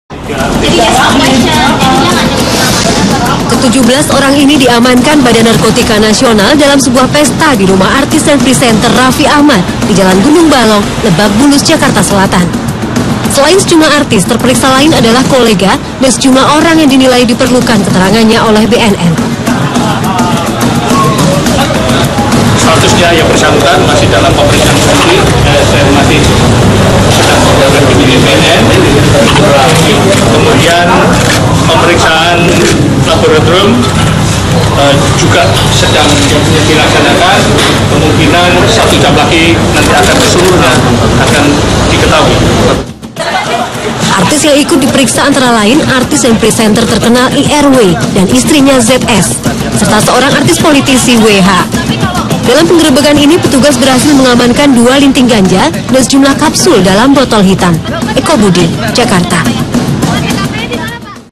BNN Grebek Rumah Raffi, 17 Pecandu Diamankan
Badan Narkotika Nasional BNN Mengdrebek kediaman Raffi Ahmad.
Dalam penggerebekan BNN juga menangkap 17 Orang lainnya termasuk artis dan anggota DPR.
Sebegitu bodohkan artis dan pejabat kita yang terjerembak dalam jurang narkoba ?